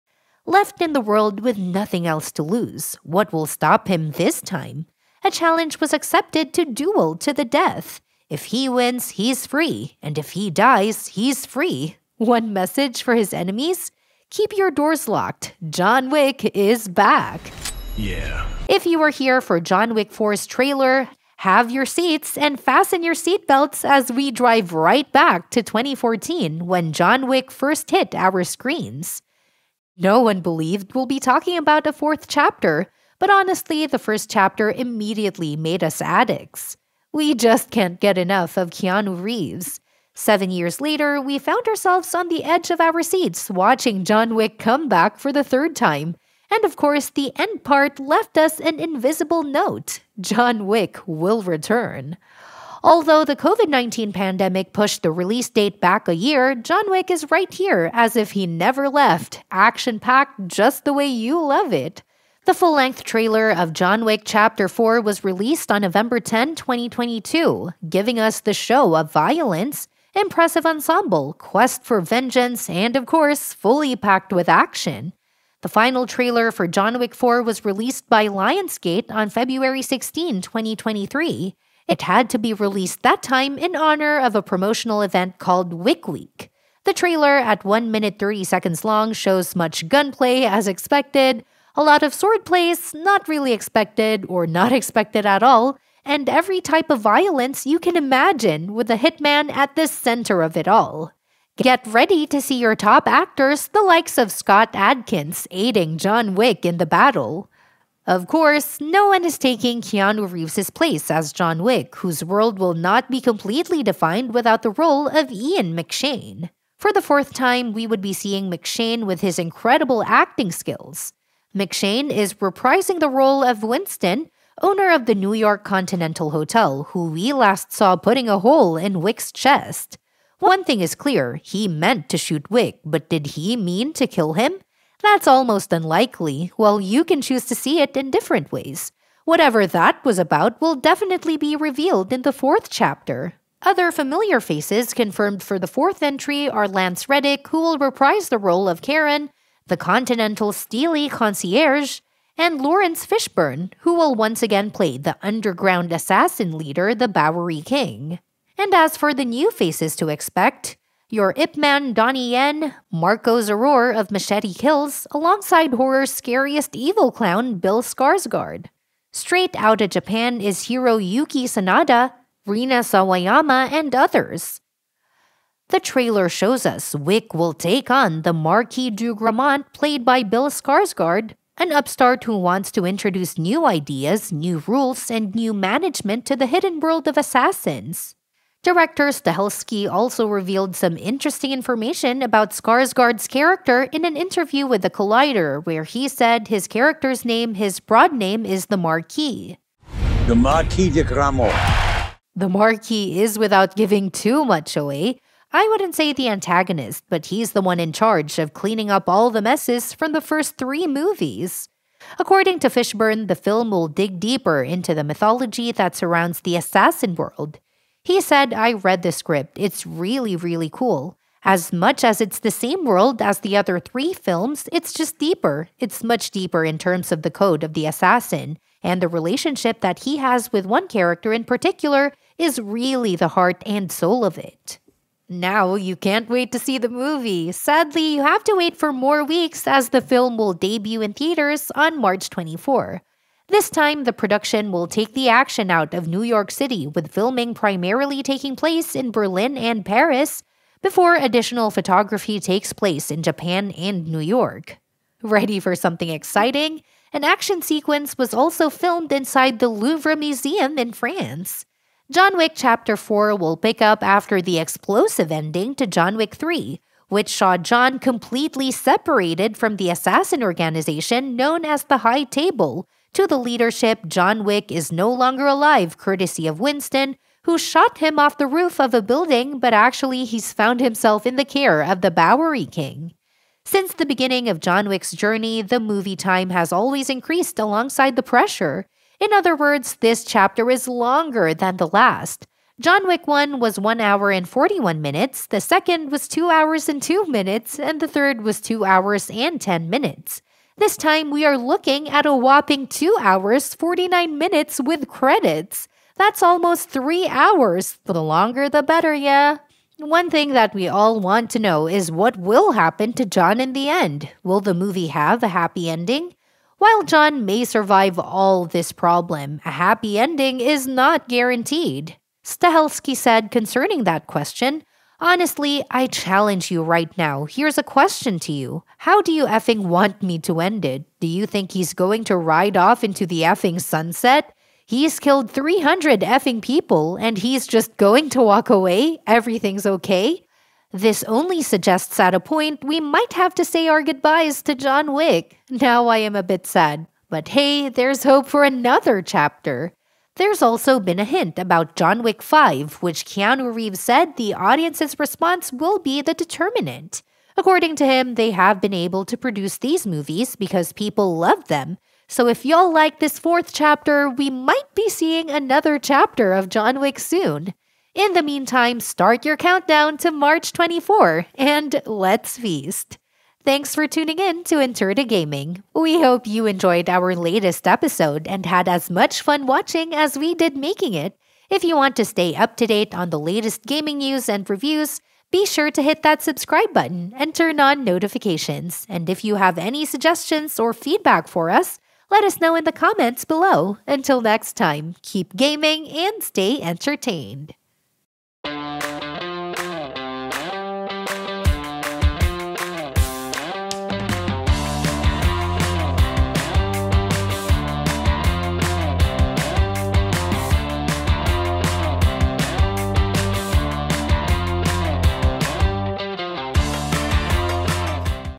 John Wick 4 - Trailer EXPLAINED
Welcome to EntertaGaming! In this video, I'll be breaking down the trailer for the highly anticipated movie John Wick 4. John Wick 4 is an upcoming action film directed by Chad Stahelski and starring the one and only Keanu Reeves. The movie follows the story of the original movie John Wick.
In this trailer breakdown, we will be discussing the key plot points, character introductions, and cinematography techniques used in the trailer to give you a better understanding of what to expect from the movie. From the opening shot to the final scene, we'll analyze every aspect of the trailer and give our thoughts on what it could mean for the overall story.
So if you're a fan of the John Wick series, or just curious to learn more about John Wick 4, make sure to stick around and watch the entire video.